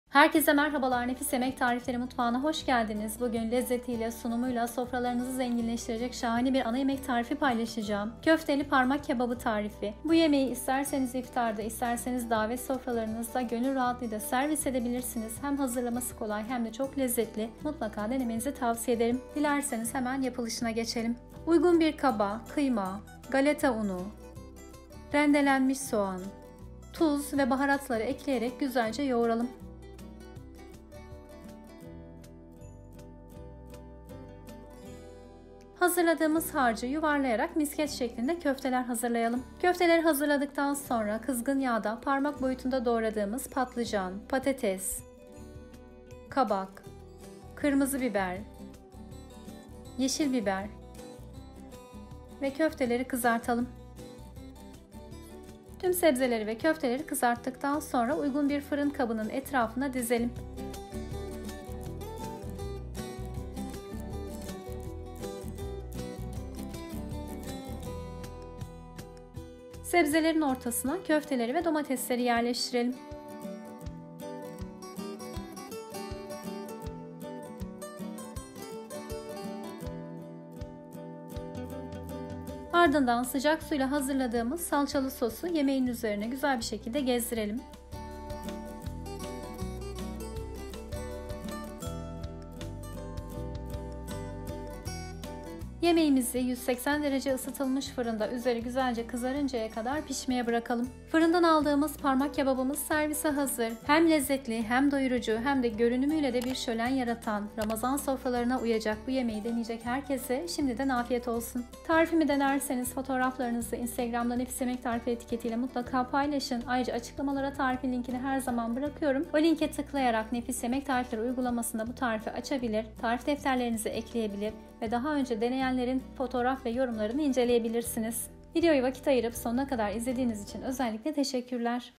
Şıklığı ve Lezzetiyle Gururla Sunmalık Köfteli Parmak Kebabı 😋| Akşama Değişik Ne Pişirsem? #5
Akşam yemeğinde görsel bir şölen sunmak isterseniz, görüntüsüyle iştah açan köfteli parmak kebabı tarifimize buyurun! 😍 Üstelik yapımı öyle kolay ki ilk defa yapıyor bile olsanız aşçılara taş çıkartacağınızı garanti ediyoruz 👌🏻😉

Köftenin sebzelerle buluşmasıyla ortaya çıkan enfes lezzetiyle damaklarda yer edinecek bu tarifimizi sevdiğiniz sebzelerle çeşitlendirmeniz mümkün. Öyleyse köfteli parmak kebabı nasıl yapılır detaylı anlatım için buyurun videomuza. Deneyeceklere şimdiden afiyetler olsun. ❤️

Köfteli Parmak Kebabı İçin Gerekli Malzemeler

Domates

Kızartmak İçin;
4 adet patates
2 adet kabak
2 adet patlıcan
3 adet kırmızı biber
5 adet yeşil biber

Köfte için:
300 gr kıyma
1 adet rendelenmiş soğan
3 yemek kaşığı galeta unu
Tuz
Karabiber
Pul biber
Kırmızı toz biber
Kimyon

Sos için;
1 su bardağı sıcak su
1 tatlı kaşığı salça

Hazırlanışı
1.Uygun bir kaba 300g kıyma,  3 yemek kaşığı galeta unu, 1 adet rendelenmiş soğan, tuz ve baharatları ekleyip güzelce yoğuralım.
2.Hazırladığımız harcı yuvarlayarak misket köfte haline getirelim.
3.Kızgın yağda parmak boyutunda doğradığımız 4 adet patates, 2 adet kabak, 2 adet patlıcan, 3 adet kırmızı biber, 5 adet yeşil biber ve köftelerimizi kızartalım.
4.Sebzeleri uygun bir fırın kabının etrafına dizelim. Ortasına köfteleri ve domatesleri yerleştirelim.
5.Ardından hazırladığımız salçalı sosu yemeğin üzerine eşit şekilde gezdirelim
ve önceden ısıttığımız 180°C fırında üzerleri kızarıncaya kadar yaklaşık 20 dakika pişirelim.
6.Fırından aldığımız parmak kebabımız servise hazır!

Uygulamamızı İndirin;